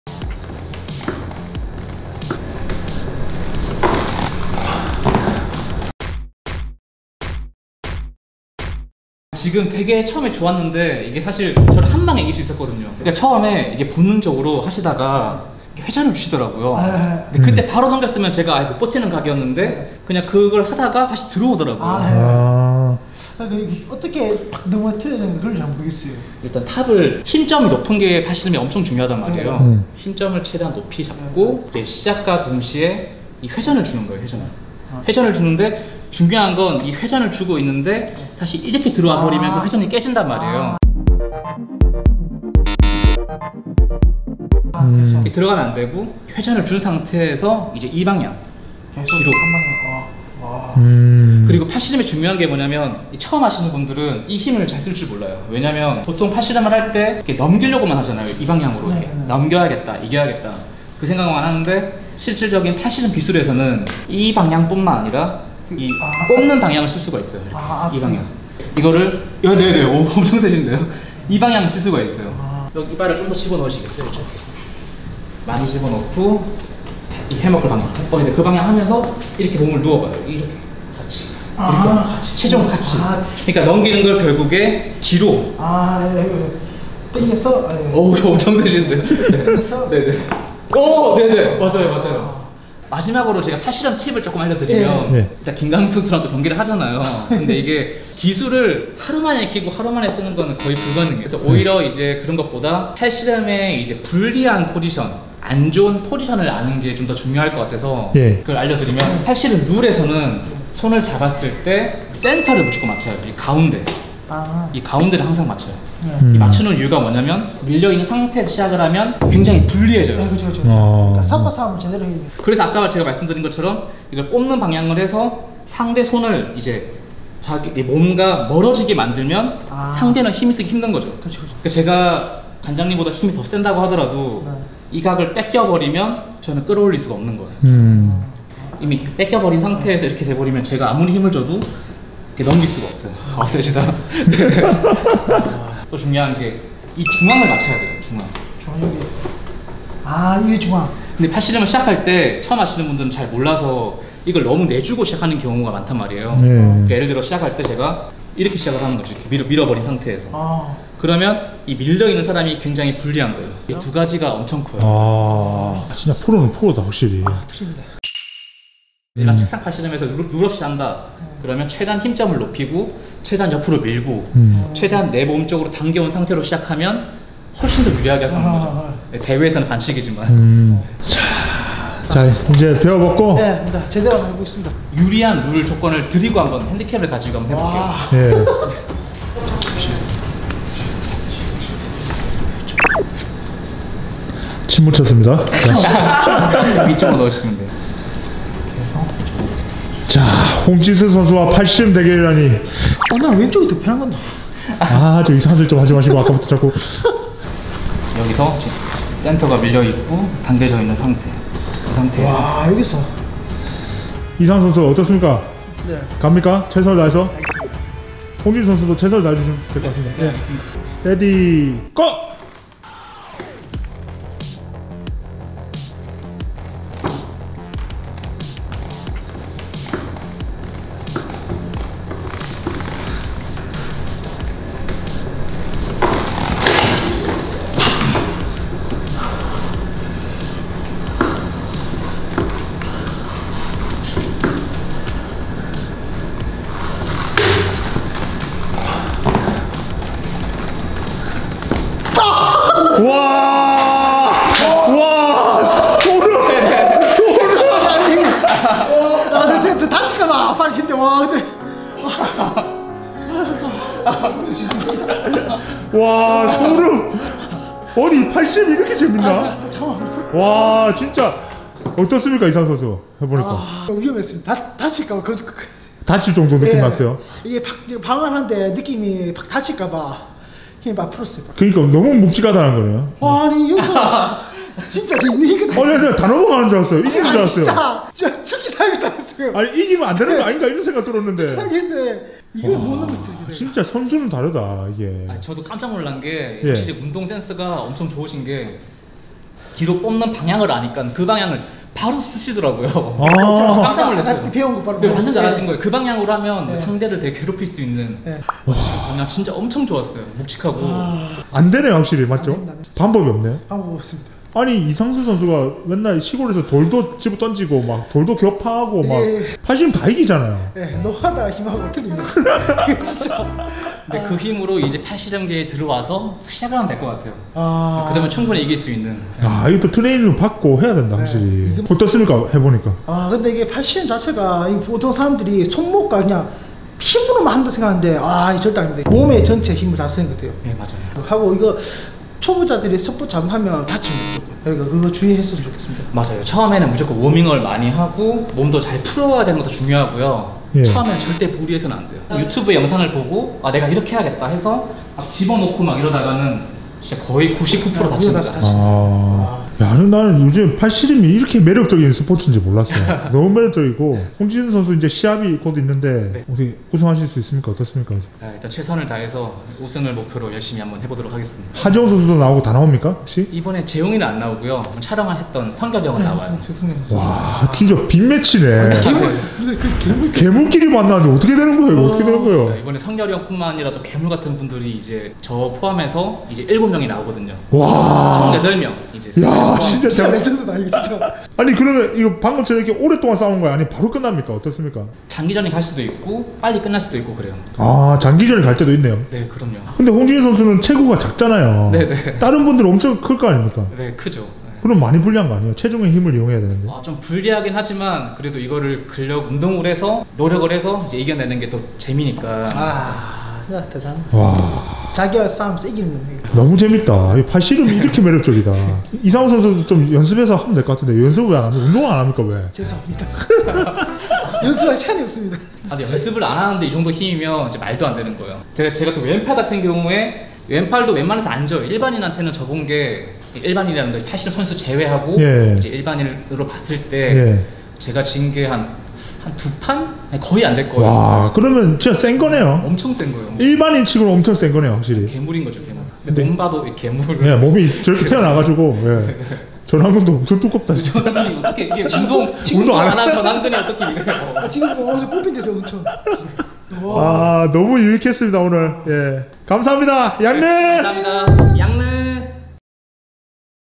팔씨름 챔피언 홍지승에게 도전하는 일반인 최강자!
팔씨름 챔피언 홍지승 오른팔을 넘기는 이상수?!
홍지승이 알려주는 팔씨름 잘하는법?!

*양성훈 감독은 부산 대신동 팀매드 본관에 계십니다  *
본관 : 부산시 서구 구덕로 334번길 5